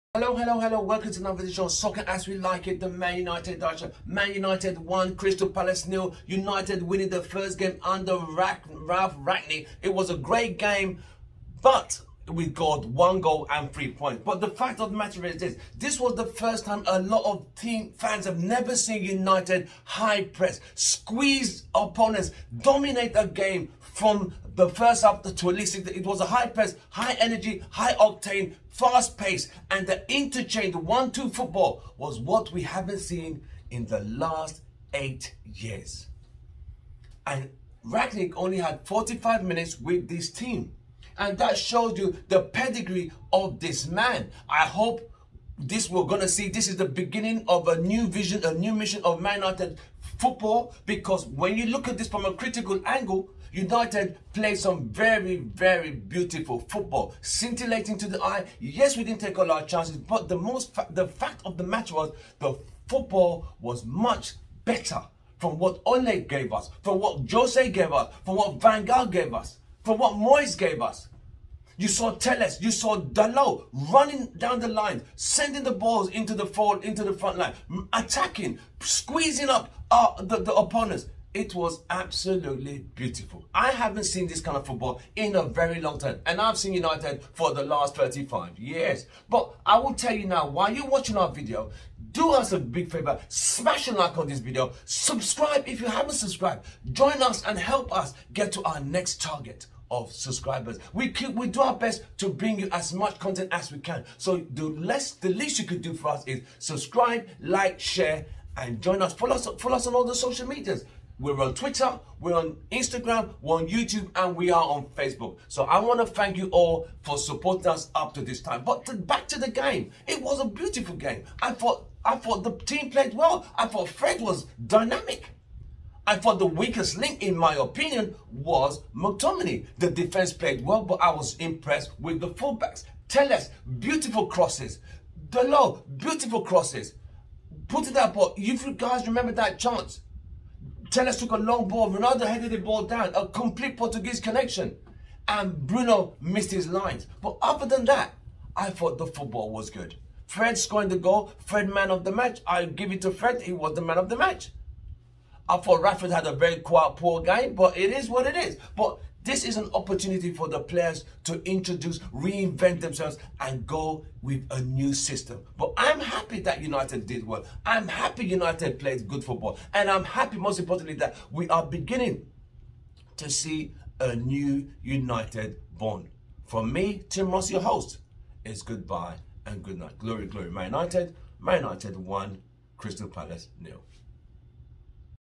MANCHESTER UNITED 1 - 0 CRYSTAL PALACE - FINAL WHISTLE
FRED IS MAN OF THE MATCH

“We love you Rangnick, we do,” sang a jubilant Stretford End near the final whistle as Fred was the unlikely hero of this deserved victory for Manchester United’s interim manager Ralf Rangnick in his first game in charge.

The winner was a smooth mid‑range finish from the maligned Brazilian – with his lesser-used right foot – and came from a cute pass from Mason Greenwood, on for Jadon Sancho, who had been United’s best performer

A delighted Rangnick said: “I had to ask my assistant coach if that was Fred’s right foot. I thought he could only shoot with his left. I’m happy for him.”

Fred displayed a poise in front of goal that had been increasingly absent as what began as a promising display meandered after the interval. Moments before Fred’s goal, United had a heart-stopping moment when James Tompkins’ knockdown from Tyrick Mitchell’s corner was hit wide by Jordan Ayew from a tight angle but with David de Gea’s goal gaping.

United are four matches unbeaten, three of them victories, and with this also only a second clean sheet of the campaign here was a perfect result for Rangnick to start his tenure.

The German’s opening XI was an if-it-aint-broke-don’t-fix-it selection that retained those who beat Arsenal 3-2. This was an intriguing move due to the glaring structural problem Rangnick had identified – a lack of control in matches – which he stated was glaringly evident in the chaotic victory here on Thursday.

The counter-view would run that Rangnick was citing a problem with style rather than personnel and that keeping the same side was smart management as the players were handed a first chance to impress.

SOURCE - THE GUARDIAN